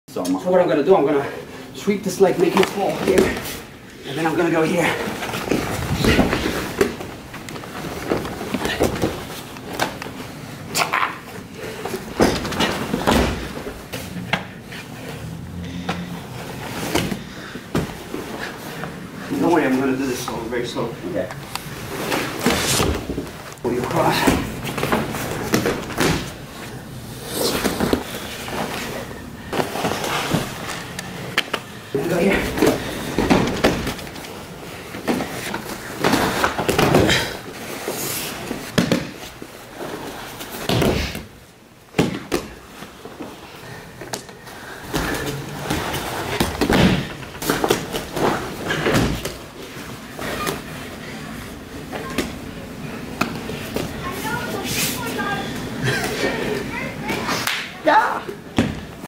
Tactics MMA Judo - Marius Enache throwing Ish Payne
Learn authentic Judo from MMA fighter, Judo and BJJ Black Belt Marius Enache